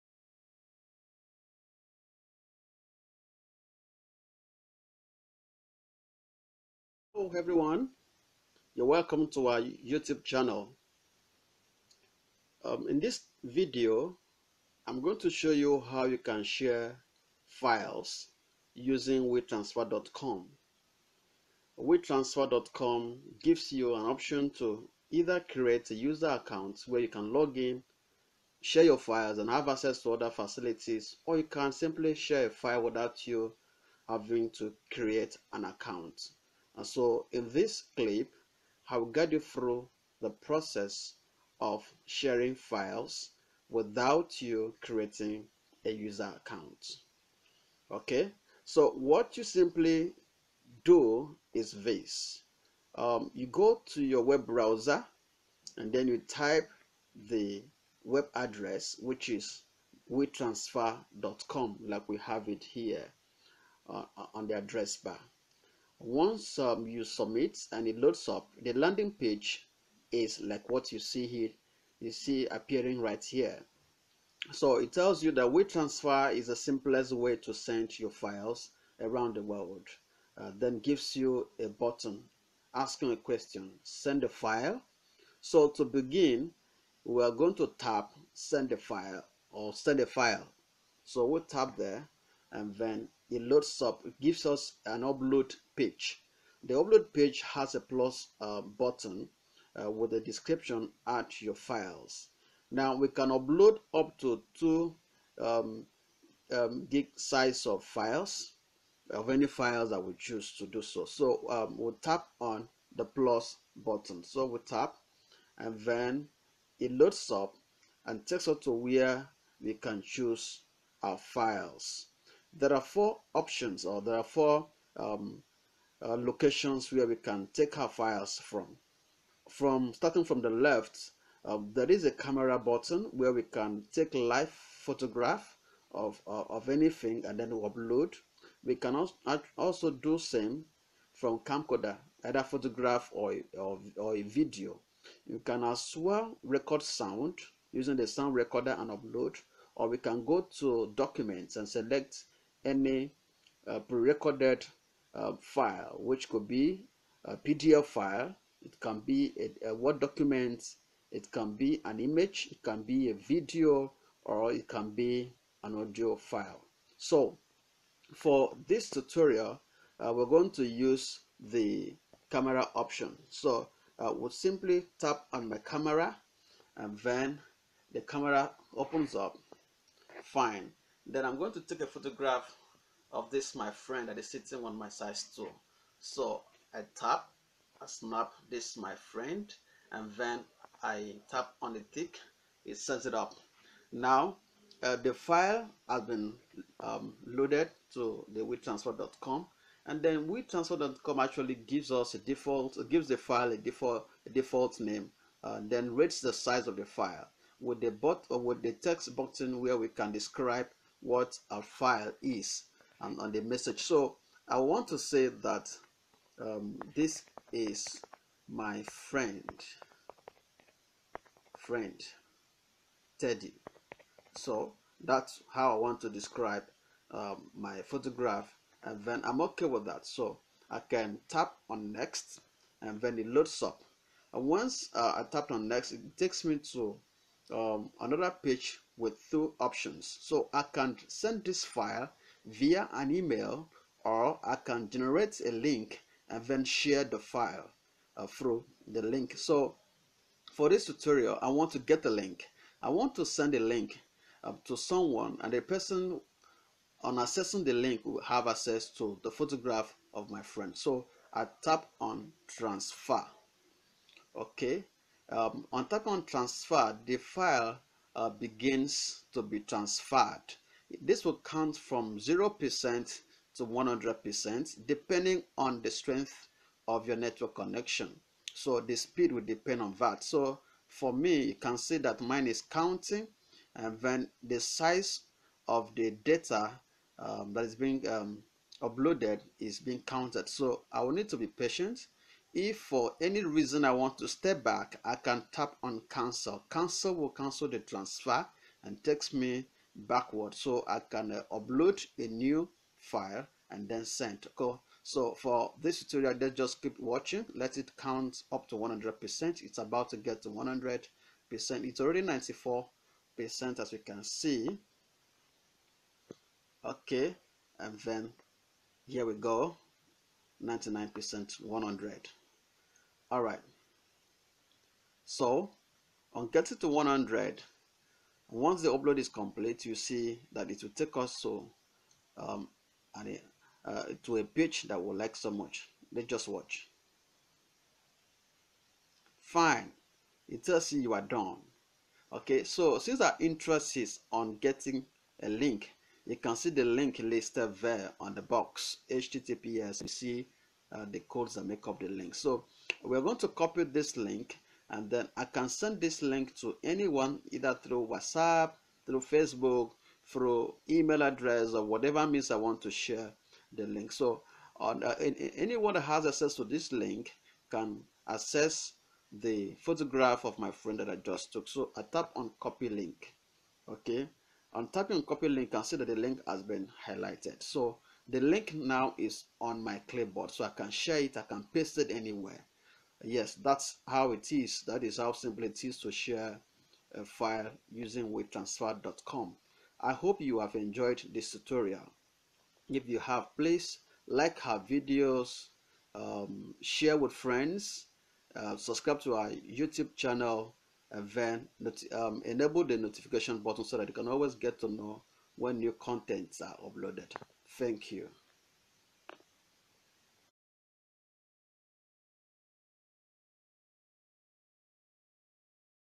How to Share Files on Phone with Wetransfer without User Account
Do you know you can share files with family, friends and colleagues in far away places without using an email? Wetransfer provides a good infrastructure for that. And you can send and receive files without even creating user accounts or downloading any app. In this tutorial, I will walk you through the process in simple steps.